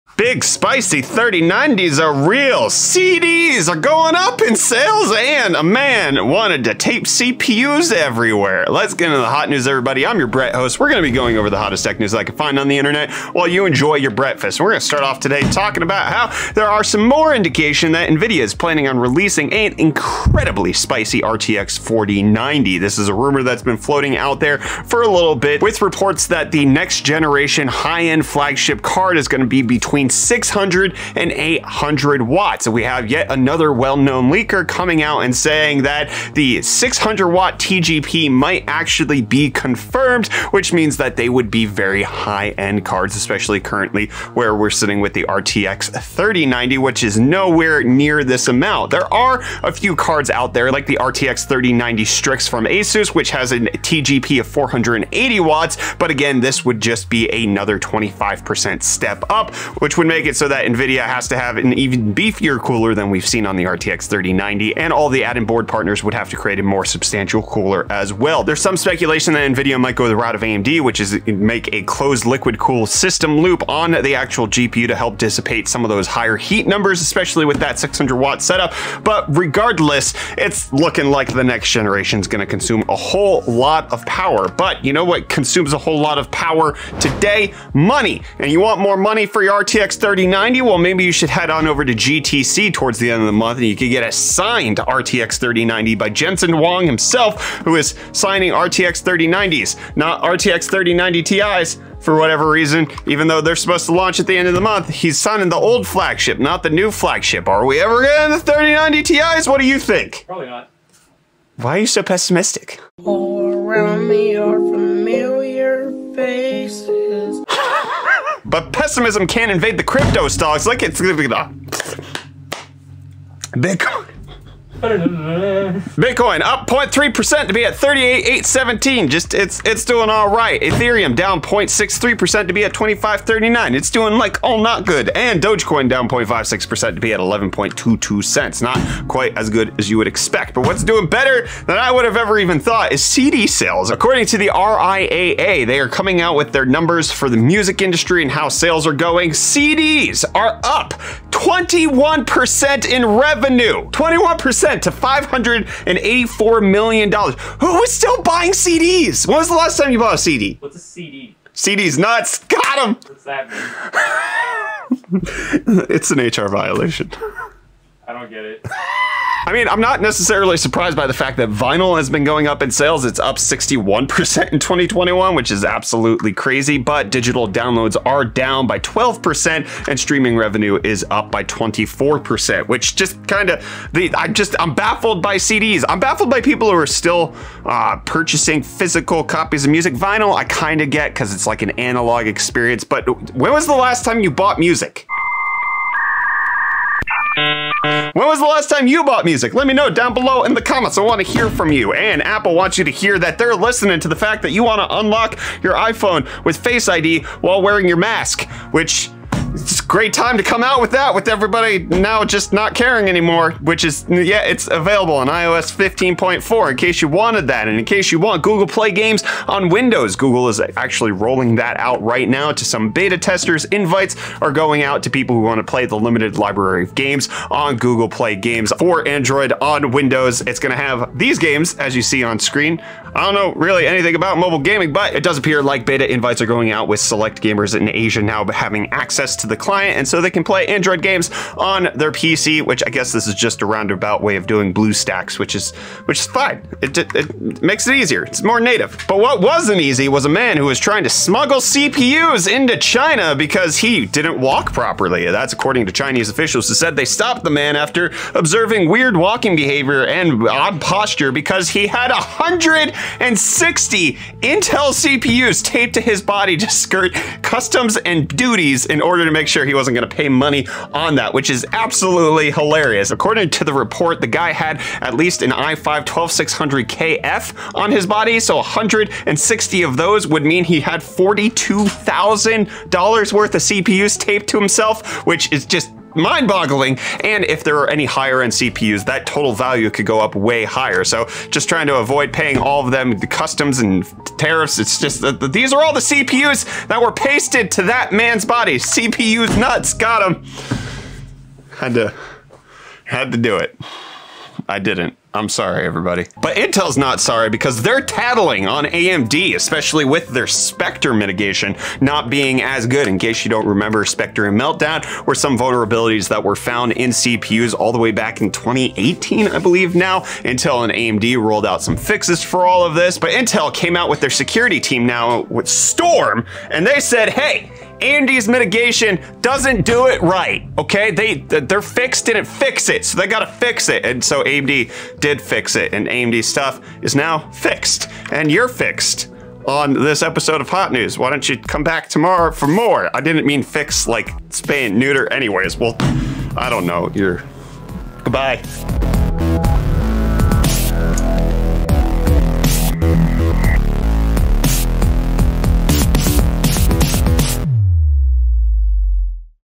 Nvidia Wants More Power
Sources & Timestamps!
0:00 - Intro
00:18 - Super Spicy 4090
01:24 - Free Signed 3090s
01:58 - Cryptostonks
02:20 - CD Sales Rose
03:21 - Mask Unlock iPhone
03:37 - Google Play on Windows
04:08 - Smuggler CPUs
05:06 - Intel Says AMD Is Vulnerable